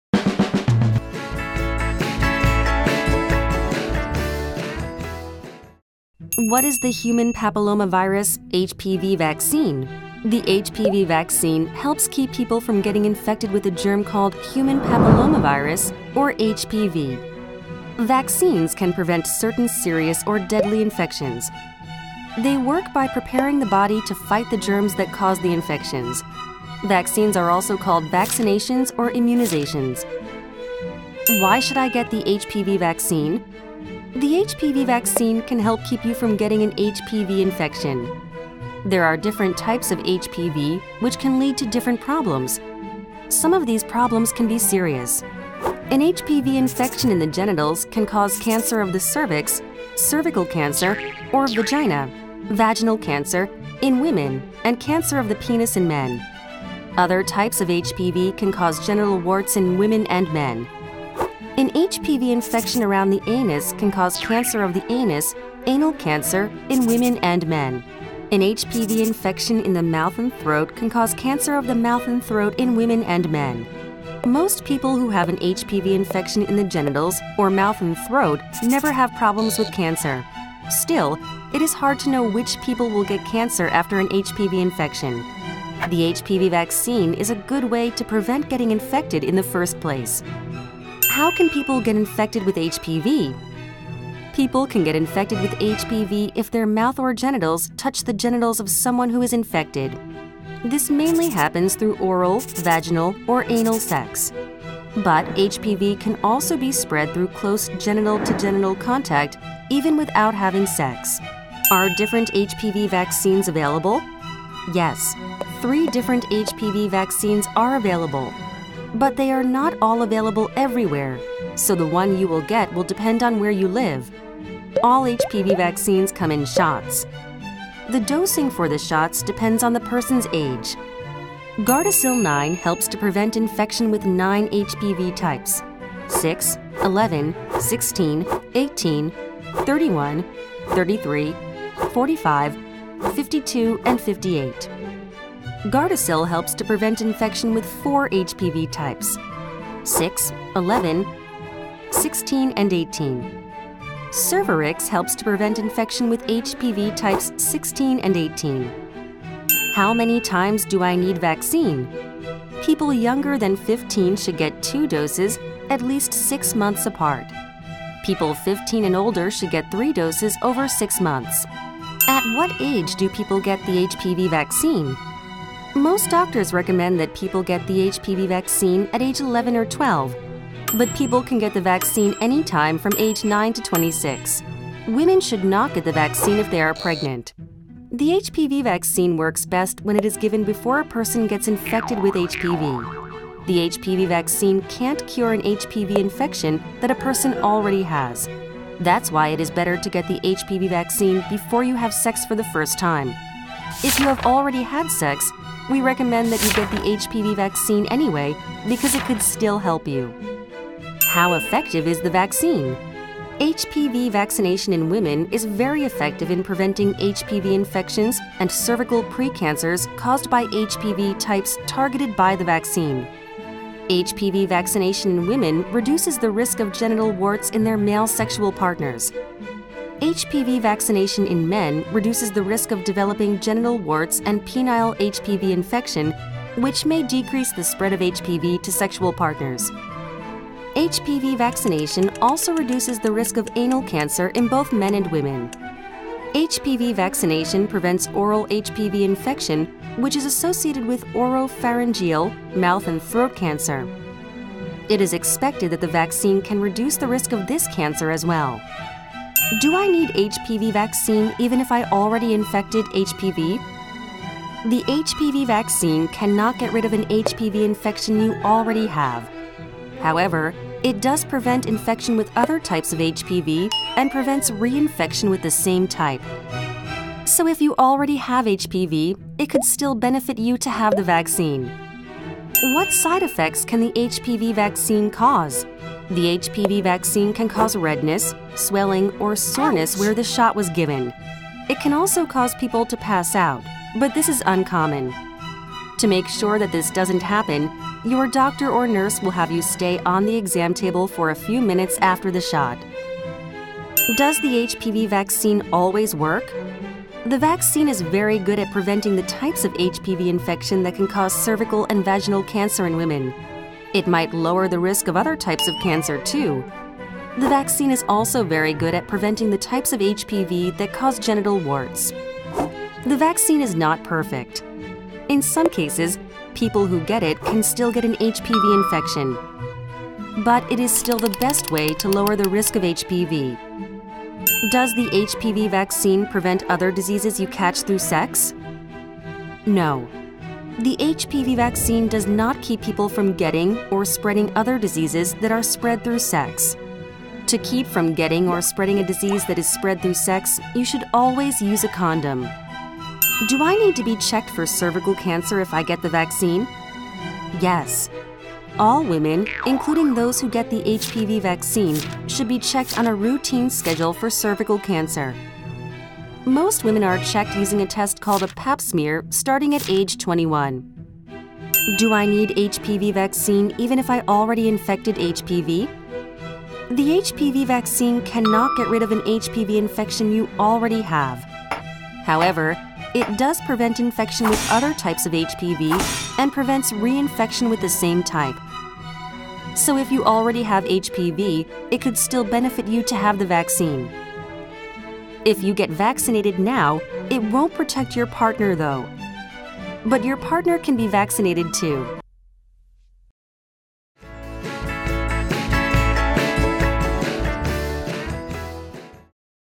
All about HPV vaccine
The HPV vaccine helps keep people from getting infected with a germ called "human papillomavirus," or "HPV." They work by preparing the body to fight the germs that cause the infections.
Today I cover below questions.
Why should I get the HPV vaccine? How can people get infected with HPV?
Are different HPV vaccines available? At what age do people get the HPV vaccine? How effective is the vaccine?  Do I need HPV vaccine even if I already infected HPV? What side effects can the HPV vaccine cause?
Does the HPV vaccine always work? Do I need HPV vaccine even if I already infected HPV?